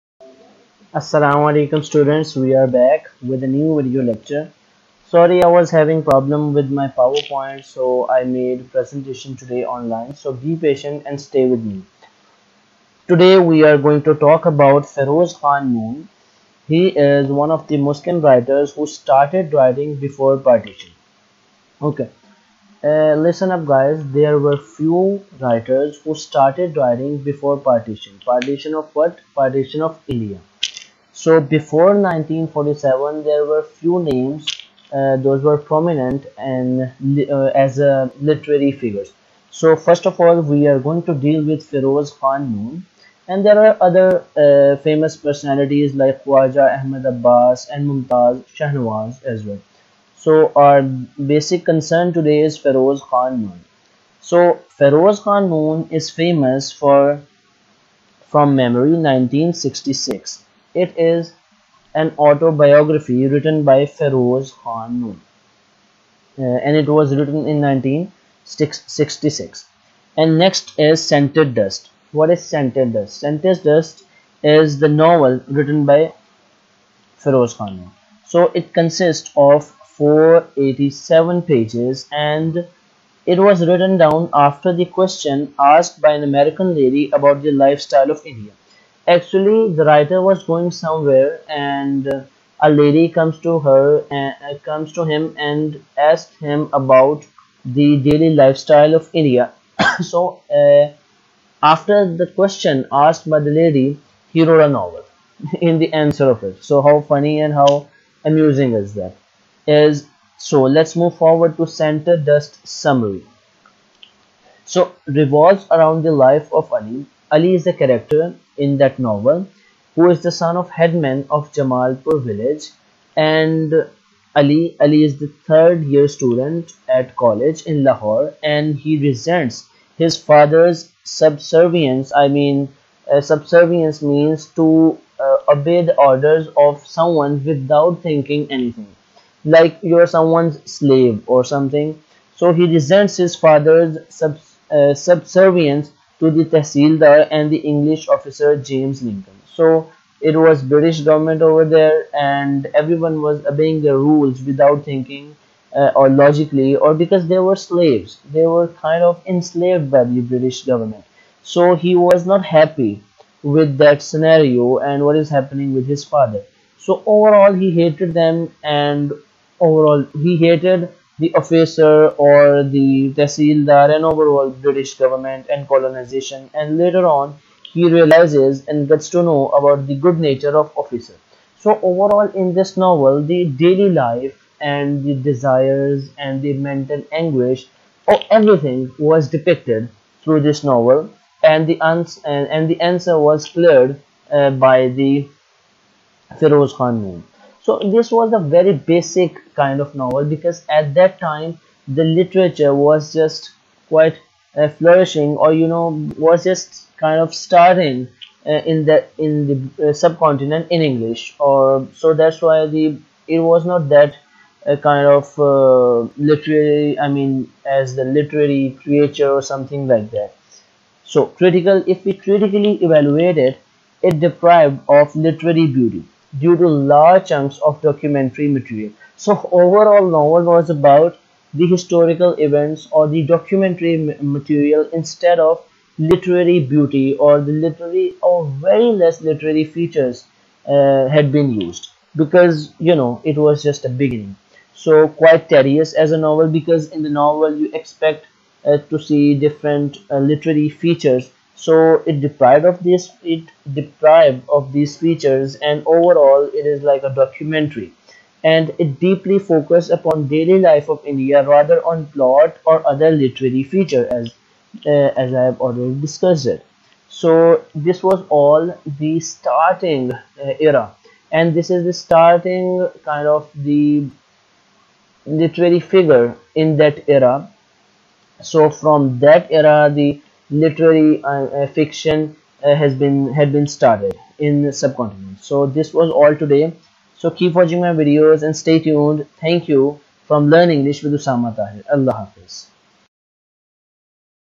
Firoz Khan Noon-Feroz Khan Noon-Writer before Partion- Pre-partition Fiction
Feroz Khan is a literary figure before partition and is famous for writing an autobiography from Memory, and a novel called scented dust. He was also once the PM of Pakistan. Let's discuss his literary works in detail.